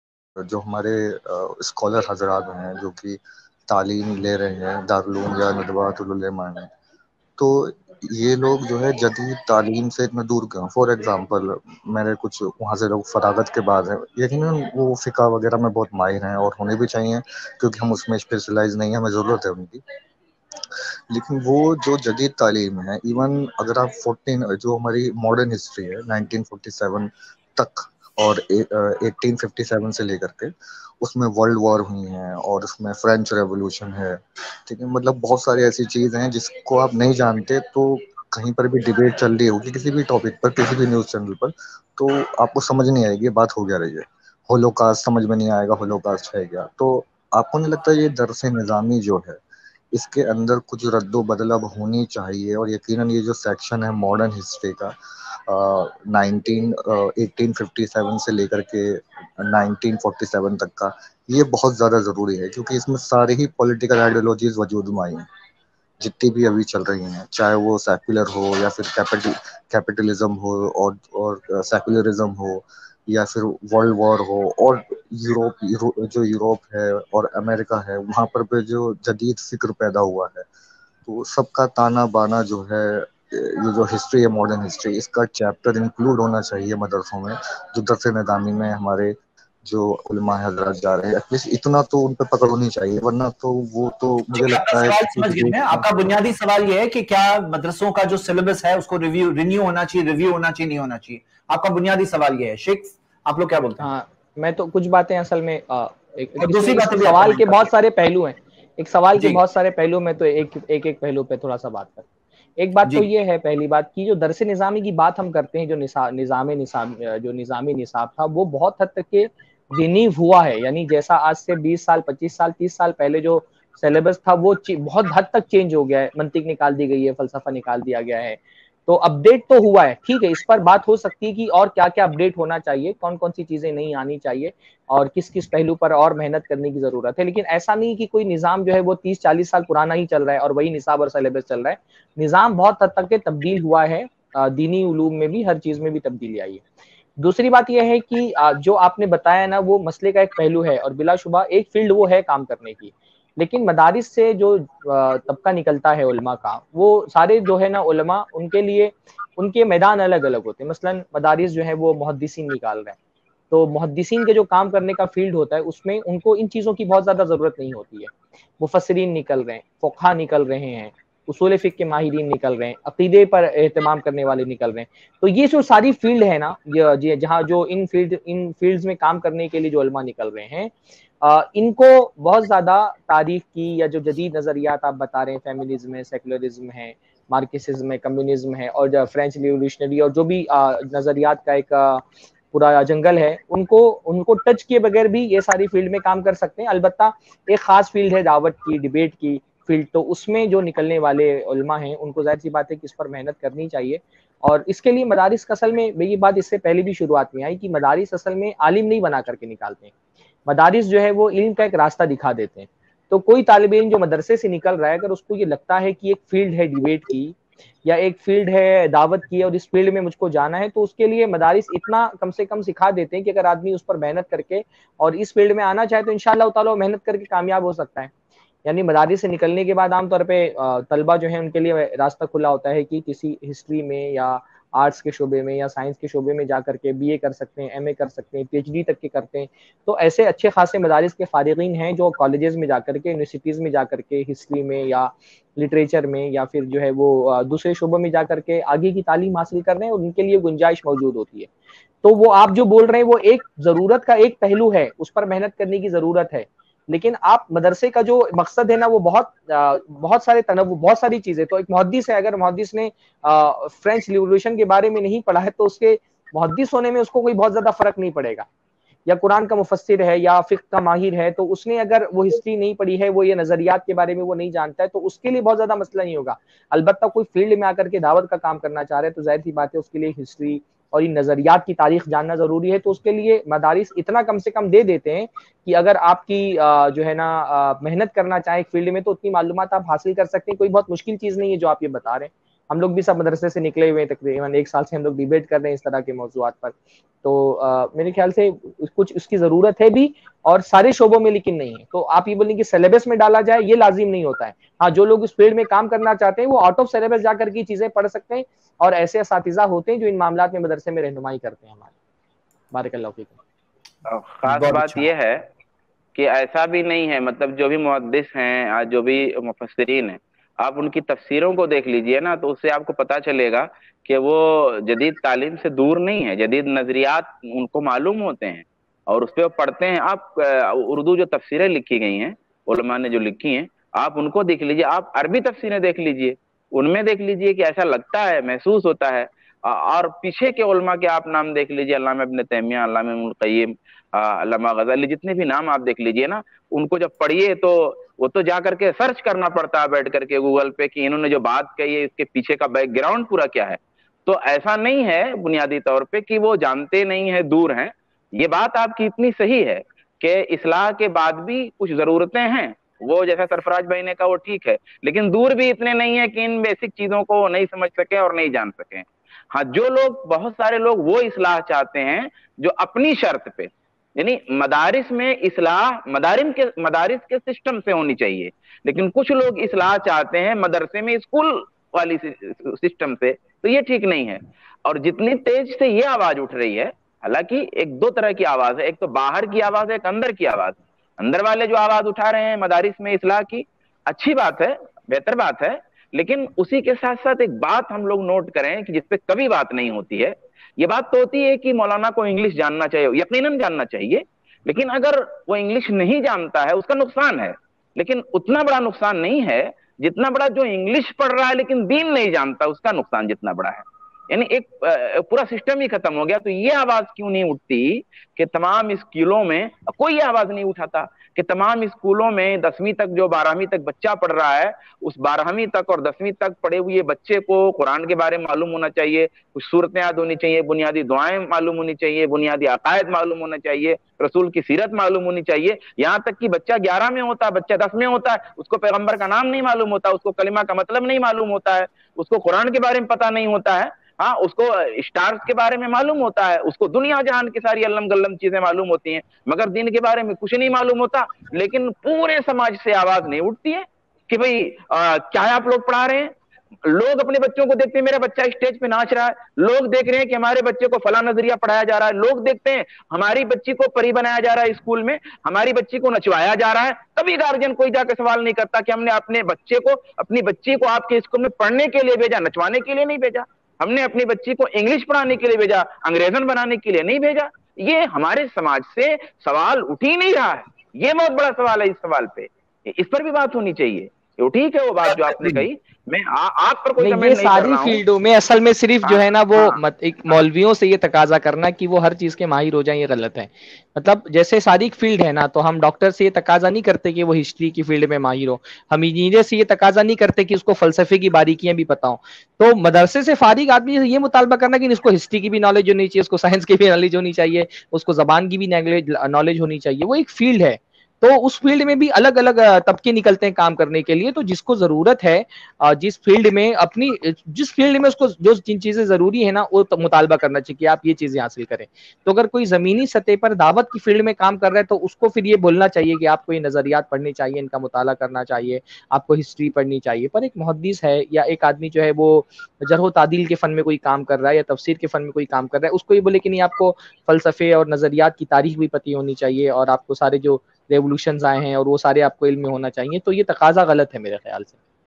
Kya Modern Zamaane Ke Hisaab Se Madarson Ka Syllebus Renew Hona Chahiye | Sarfaraz Faizi Zaid Patel
Kya Modern Zamaane Ke Hisaab Se Madarson Ka Syllebus Renew Hona Chahiye? | Shaikh Sarfaraz Faizi | Zaid Patel

UPI : donateilm@icici